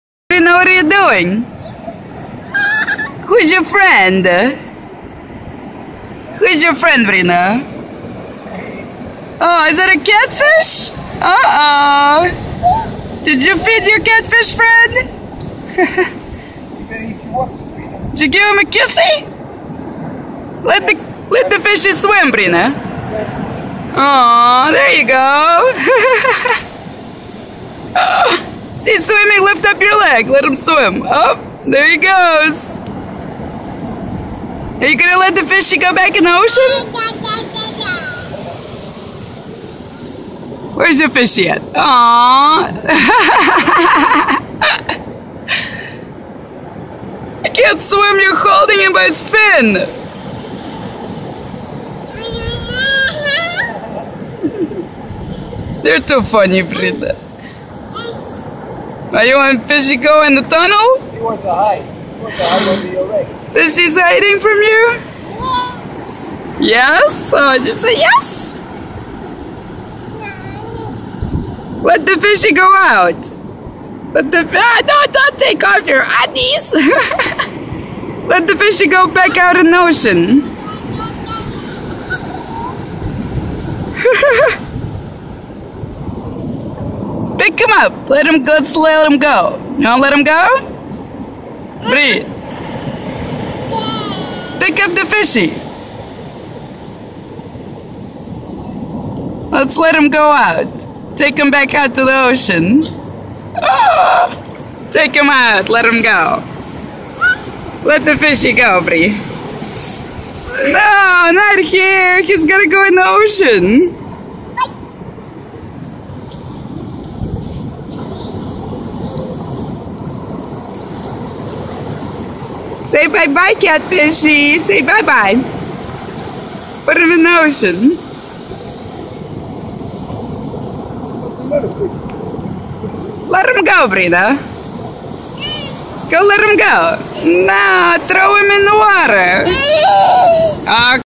But I don't wanna let the fish go..
Sabrina playing with a catfish in a little pool we dug on the beach. Sabrina is the silliest lil girl. Luv you baby!